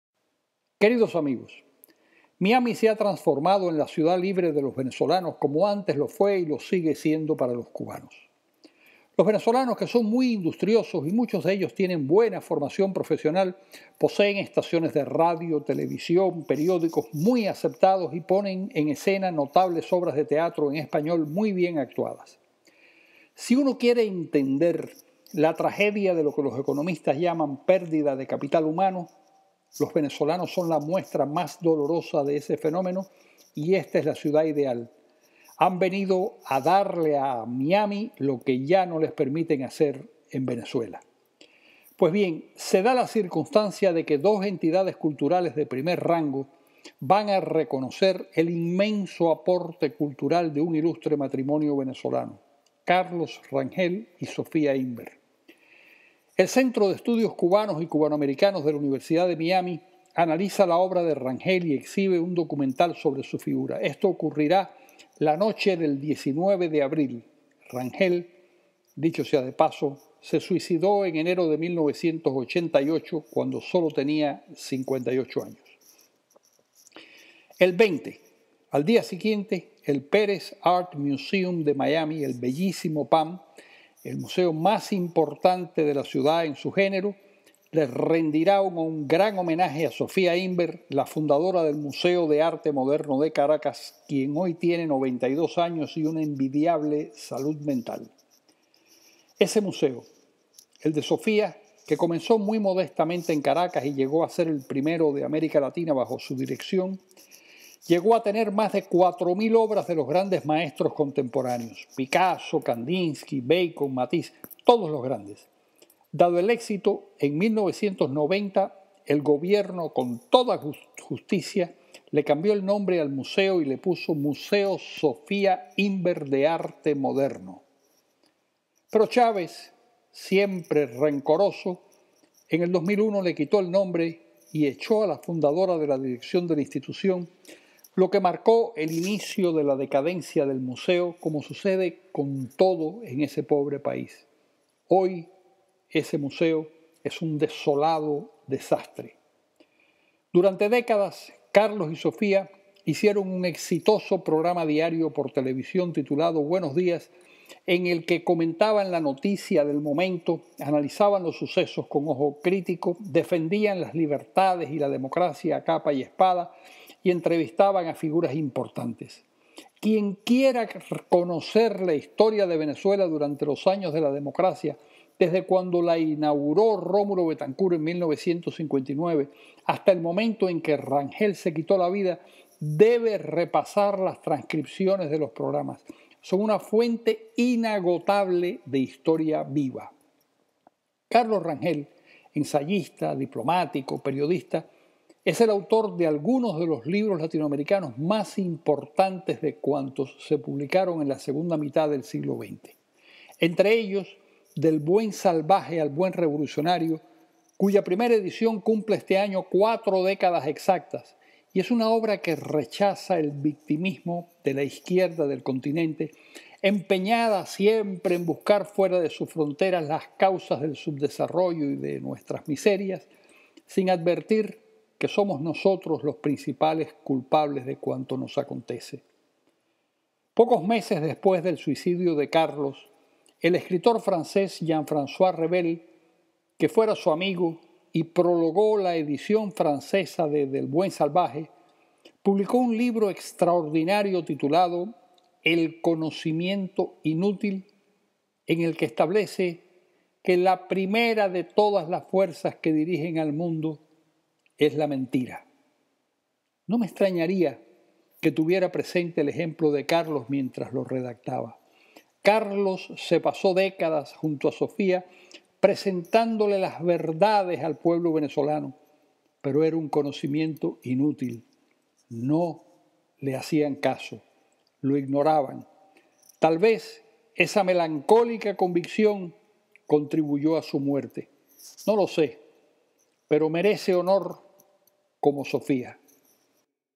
Carlos Alberto Montaner - Carlos Rangel y Sofía Imber: el homenaje debido
Carlos Alberto Montaner nació en La Habana, Cuba, en 1943. Reside en Madrid desde 1970. Ha sido profesor universitario en diversas instituciones de América Latina y Estados Unidos. Es escritor y periodista. Varias decenas de diarios de América Latina, España y Estados Unidos recogen desde hace más de treinta años su columna semanal.